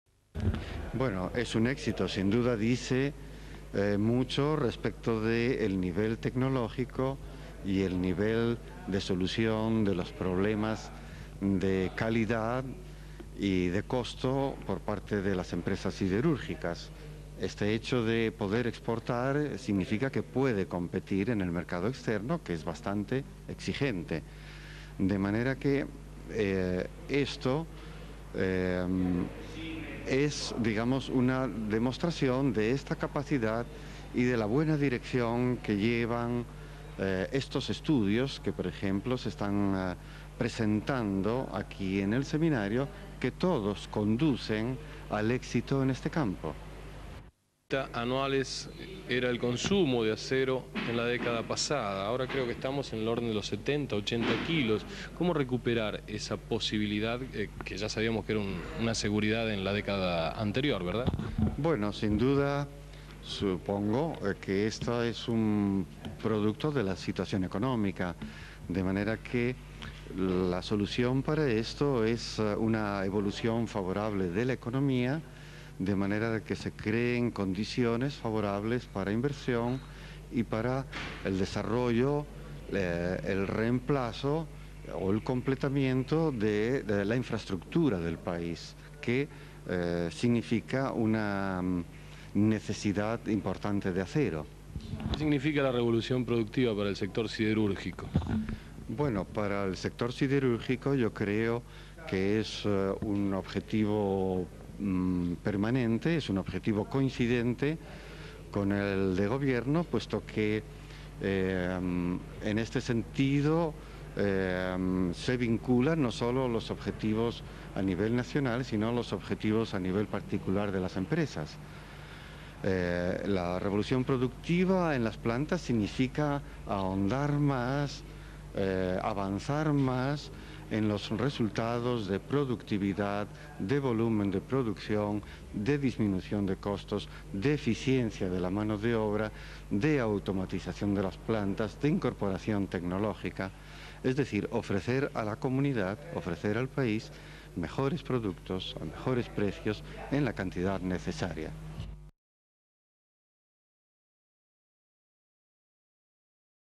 Congreso Latinoamericano de Siderurgia - DiFilm (1989)
Reportaje al señor Lopez habla de la revolución productiva venta de acero exportación.
Fecha: 15/12/1989
Duración: 2 minutos 32 segundos.
Código: UG-1514